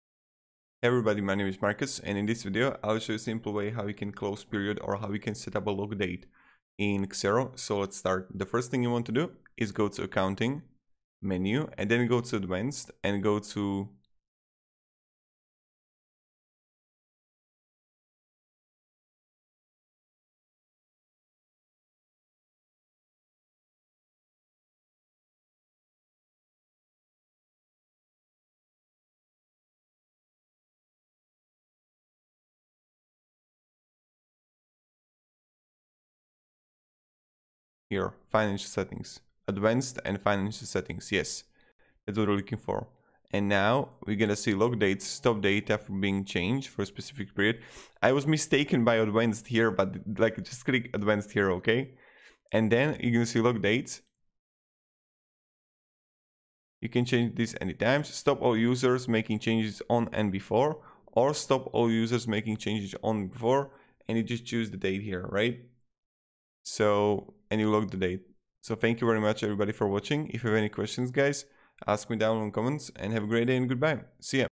How To Close Period In Xero (How To Lock Dates In Xero)
In this video I will show you How To Close Period In Xero

GET AMAZING FREE Tools For Your Youtube Channel To Get More Views:
Tubebuddy (For GROWTH on Youtube):